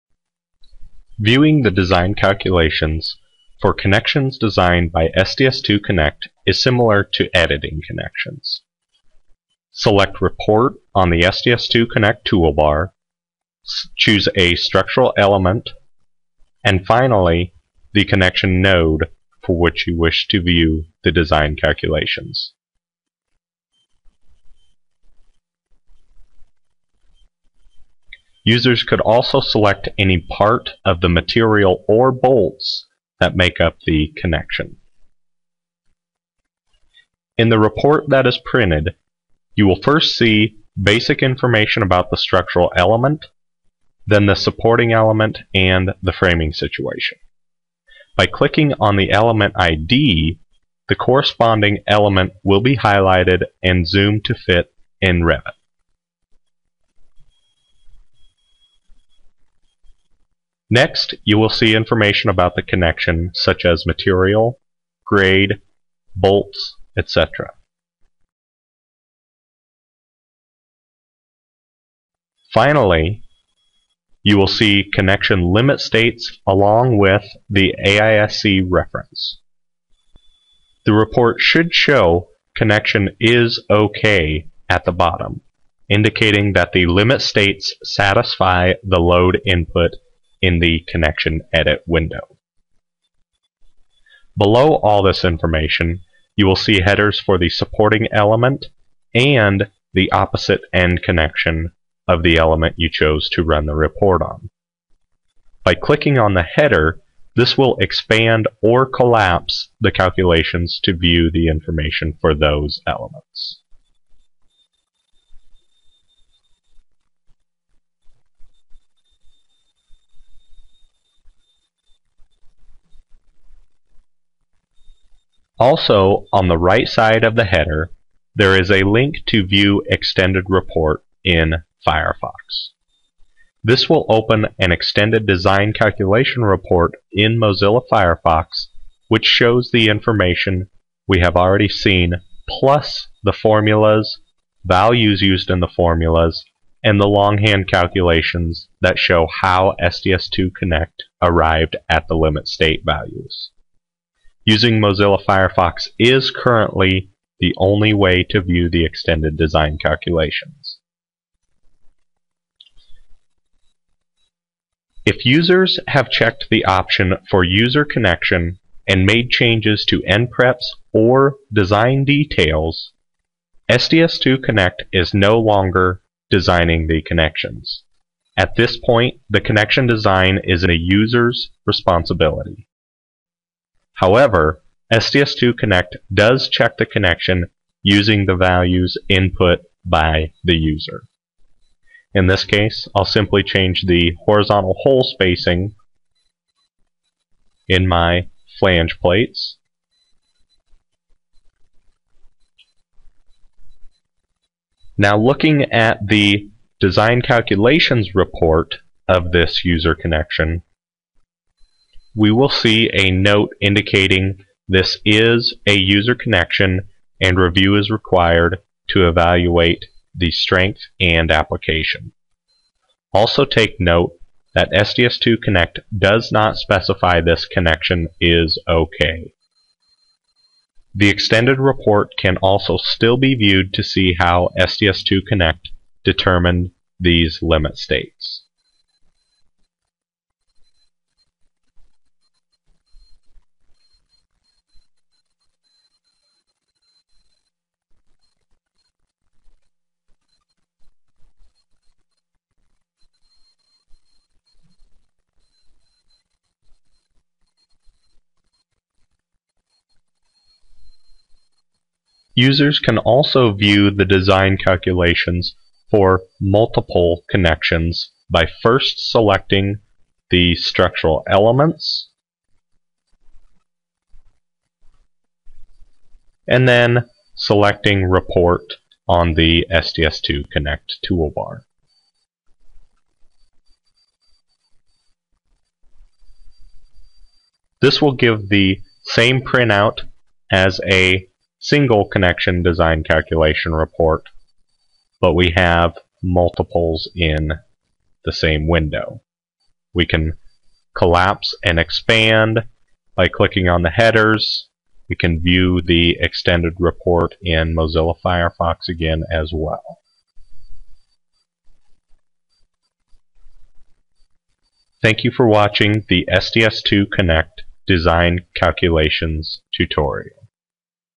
Design Calculations in Revit Structure With SDS/2 Connect
Within Revit Structure, the SDS/2 Connect add-in provides users with a report of design calculations, including the long hand calculations, which shows how it arrived at the connection results. In this video, you will learn how to use the design calculations report, and see some of the content that is provided.